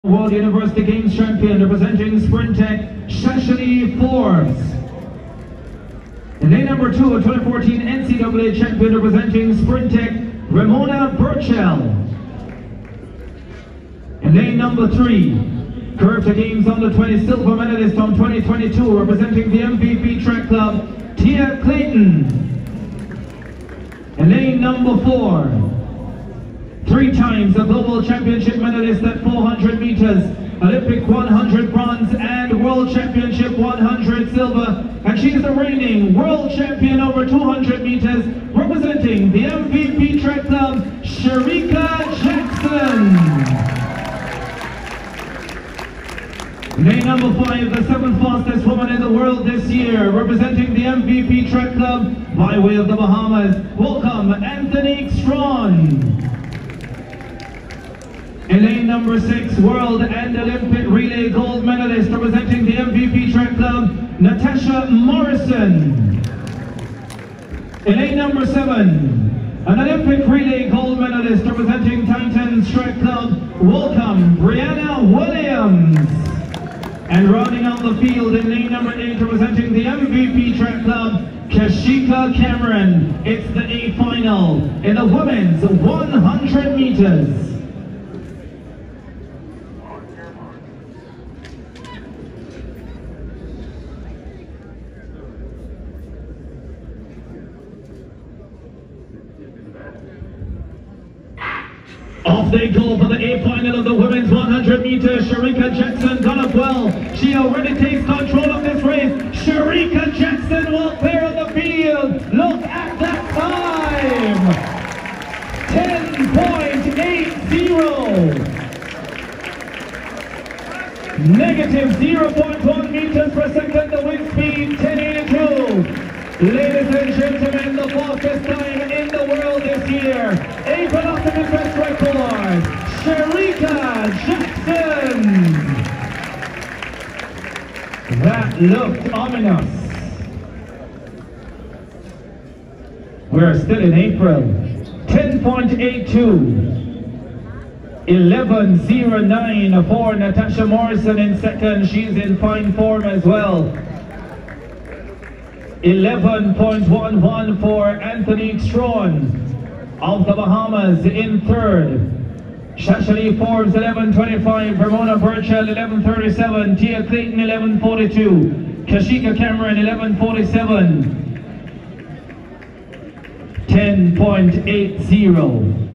Shericka Jackson runs 10.82| Velocity Fest 13
World Championship 200m gold medalist Shericka Jackson runs 10.82 seconds, a new World lead in Kingston Jamaica. The field comprised Natasha Morrison, Tia Clayton, Anthonique Strachan, Shashalee Forbes, and Remona Burchell.

Briana Williams did not show up for the final.

Elaine Thompson-Herah was slated to compete but did not show up.